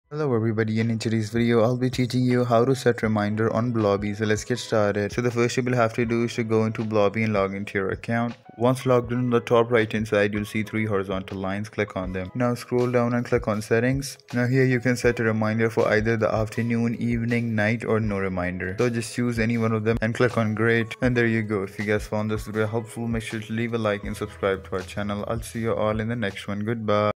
How To Set Reminder On Blobby (In 30 Seconds)
How to Set Reminder On Blobby

In this video I'll show you how to set reminders on blobby. The method is very simple and clearly described in the video. Follow all of the steps & set reminder on blobby